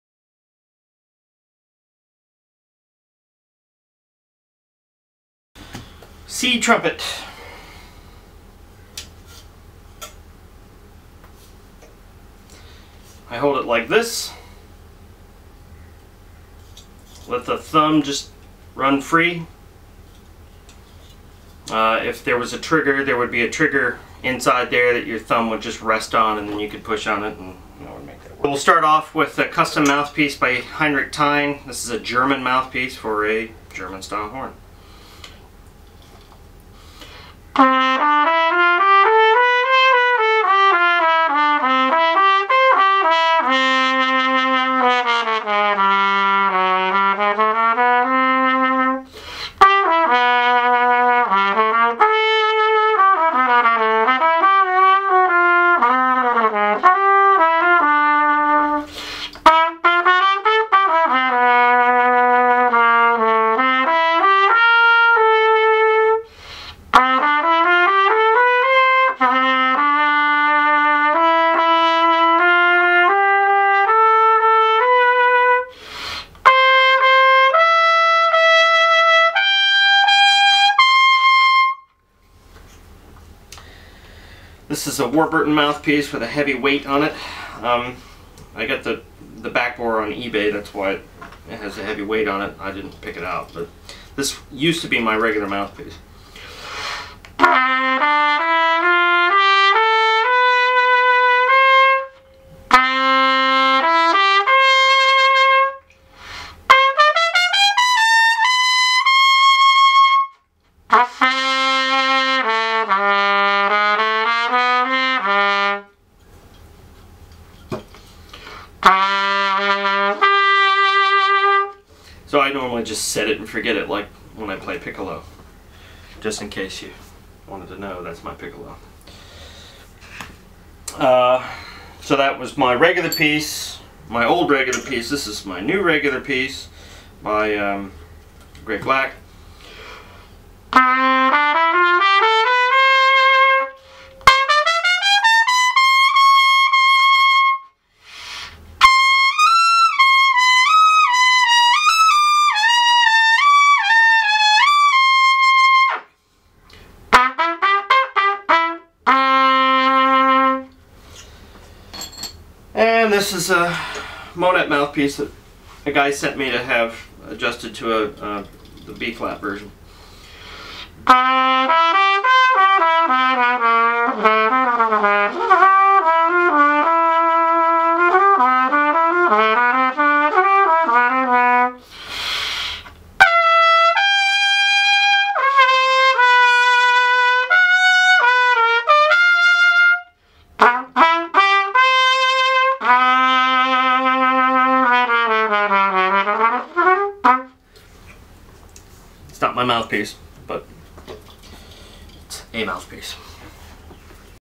George's C Trumpet
C trumpet. Gansch style, One piece drawn inside slides. Big Bell Gold Brass Bell. This is just me noodling on a C trumpet to try and get a sense of how it plays with some different mouthpieces.